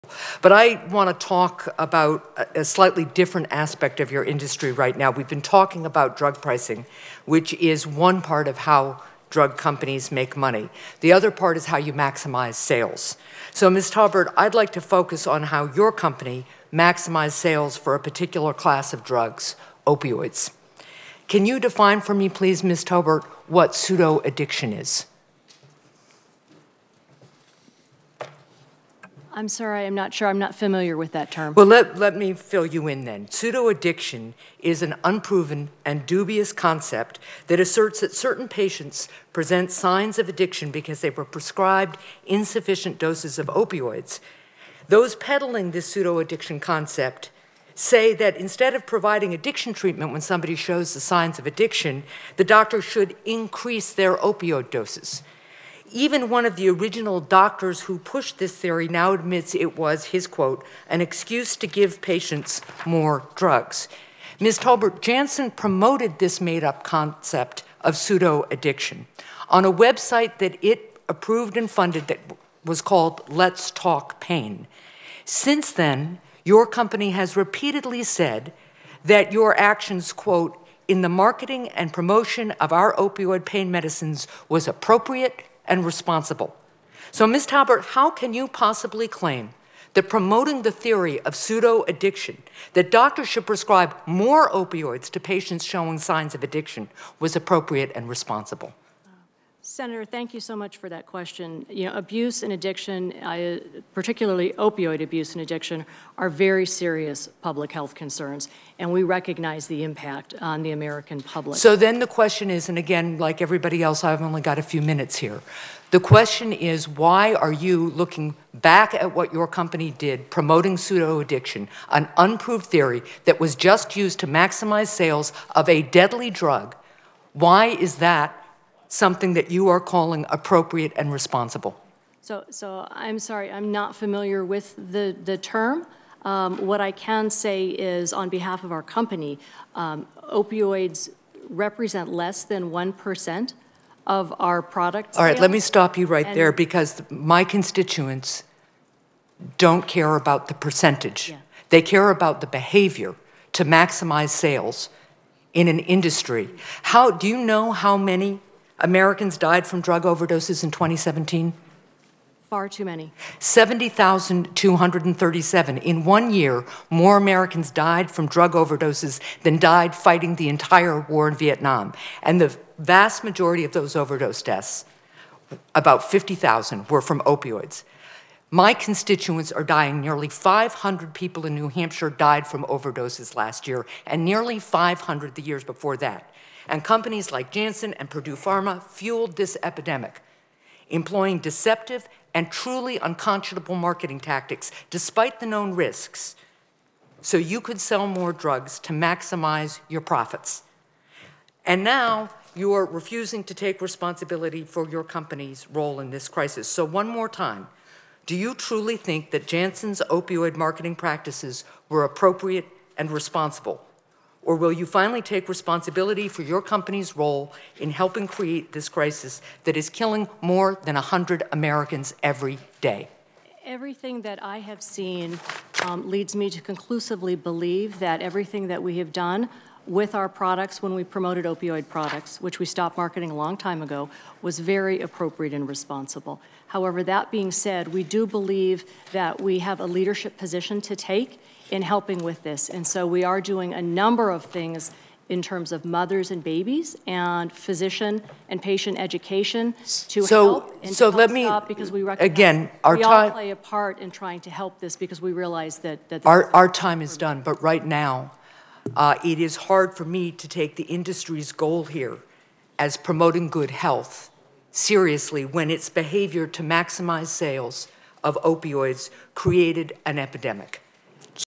WATCH: Drug companies sold opioids with 'truly unconscionable marketing tactics,' Hassan says
Sen. Maggie Hassan, D-N.H., questioned Jennifer Taubert of Janssen Pharmaceuticals about "pseudo-addiction," what Hassan said was a debunked theory used to maximize sales that claimed that patients who showed signs of addiction to opioids should be given greater doses. Taubert said she believed everything her company has done to market opioids was done appropriately.